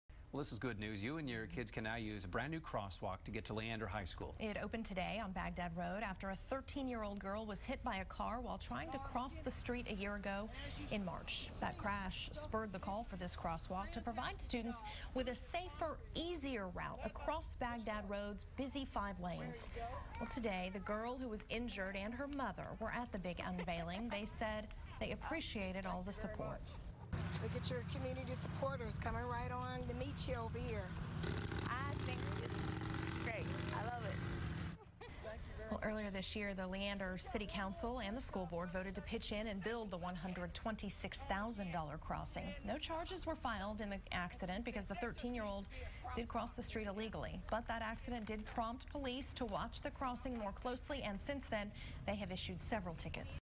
New crosswalk installed in Leander
Parents and kids can now use a new crosswalk to get to Leander High School.